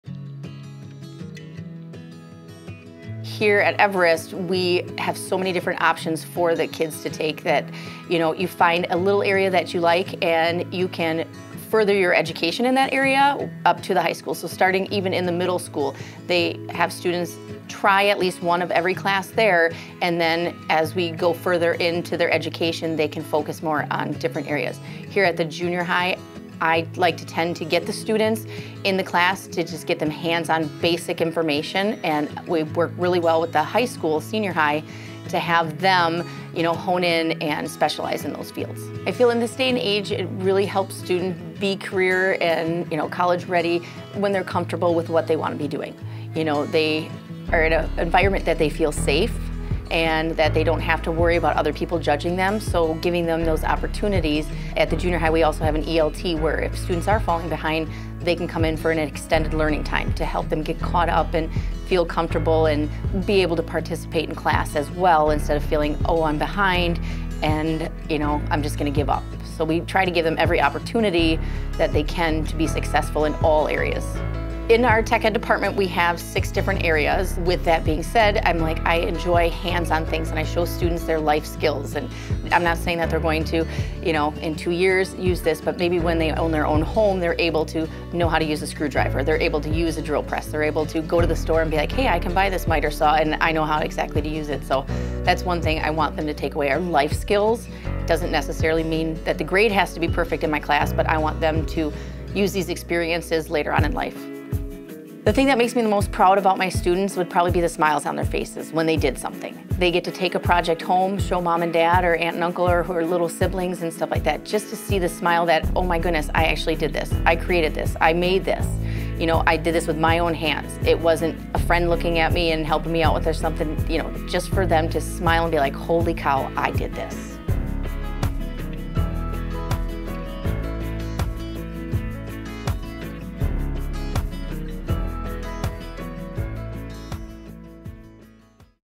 D.C. Everest - A Great Place to Learn - Junior High School
At the D.C. Everest Junior High, our students build on the skills they acquired at the DCE Middle School with hands-on learning opportunities where they can further explore their interests in preparation for advanced study at the DCE Senior High. We provide a culture where students feel comfortable taking risks — to try new things and explore emerging interests — and then help them build the academic, career and life skills they need for a successful future. Our District goals are to ensure D.C. Everest is a great place to learn, work and live.